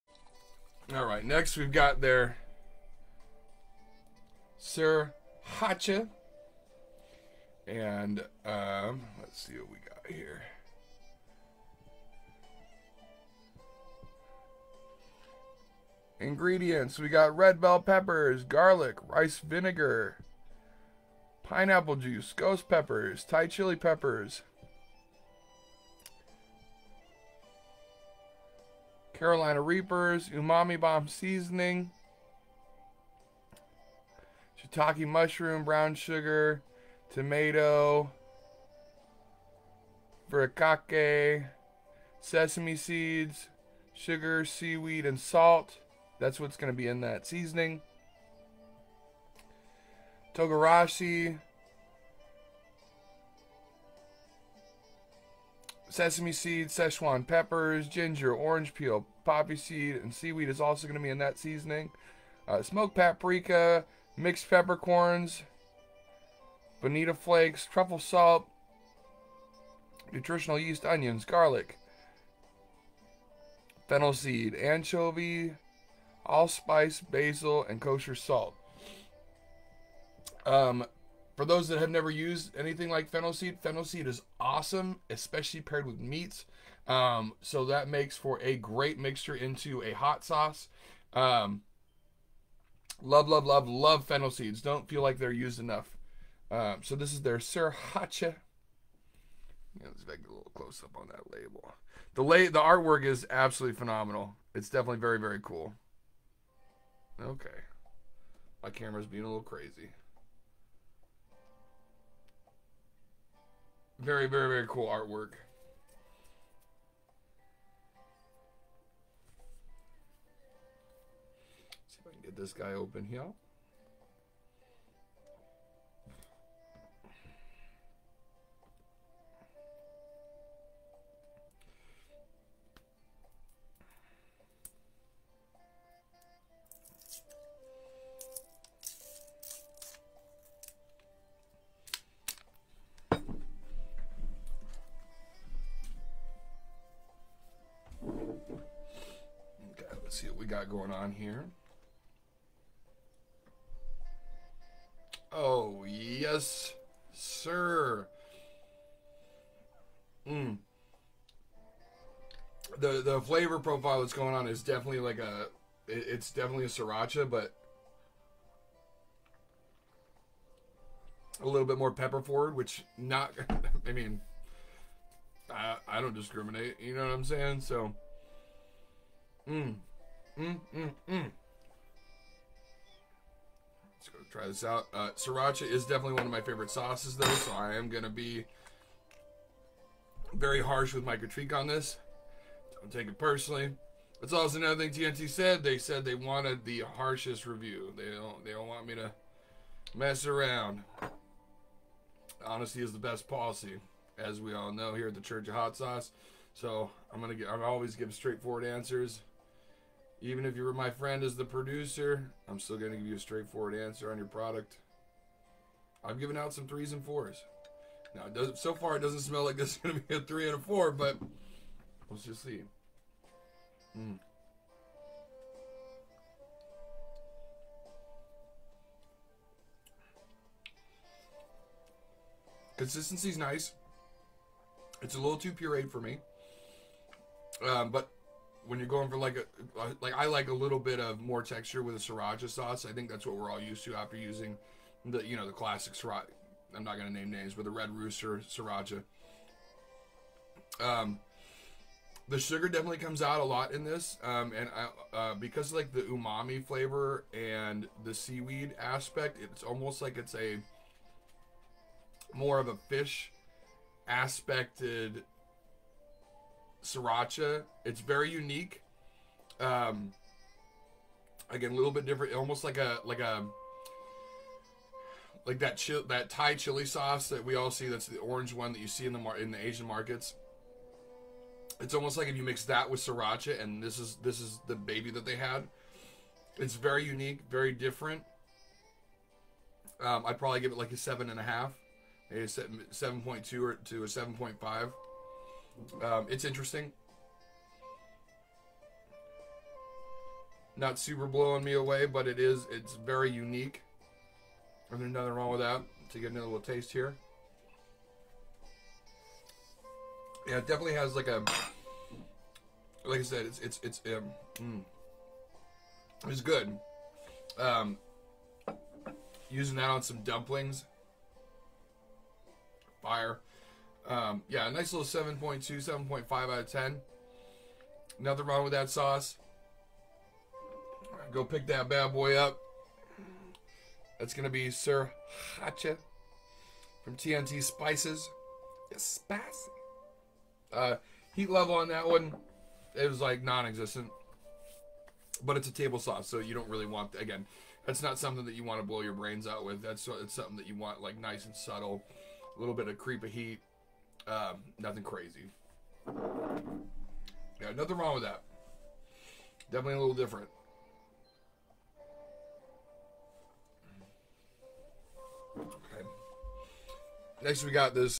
TNT Spices Sir Hot Cha Hot Sauce
A Pennsylvania hot sauce makers take on a popular style of hot sauce, sriracha.